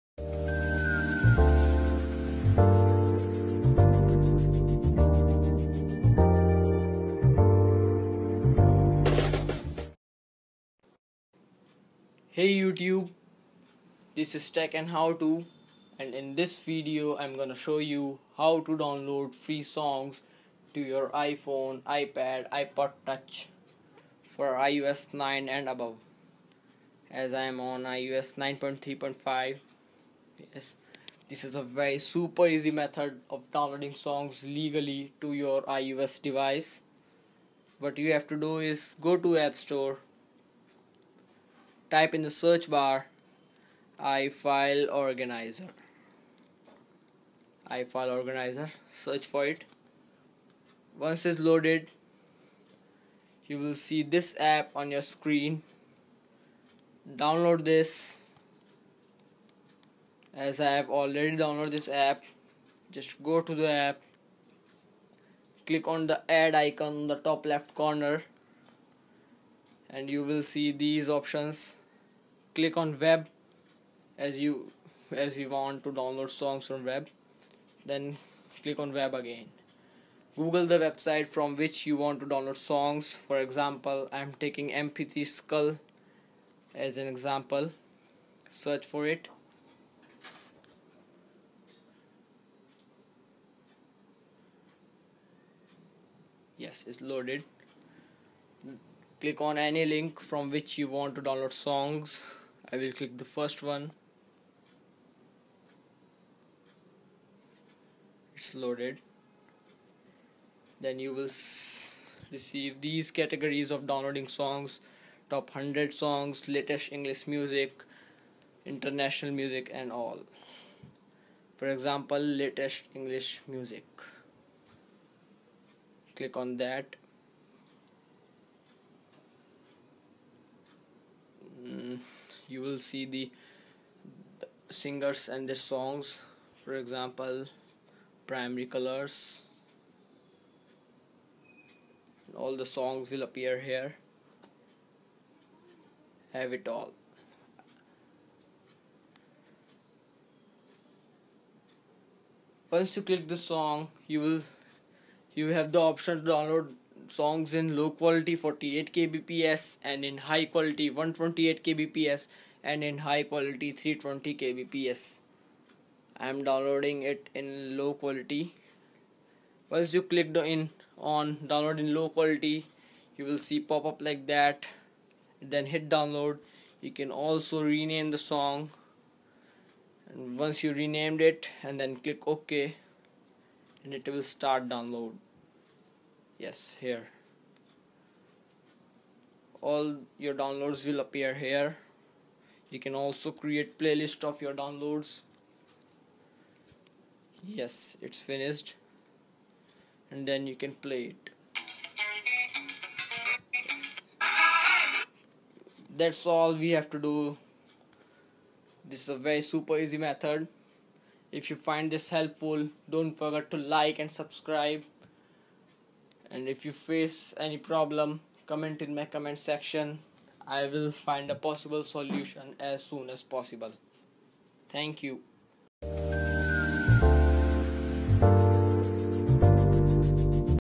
How to use IFILE ORGANIZER for downloading songs
downloading songs free and legally to your ios device  with IFILE ORGANIZER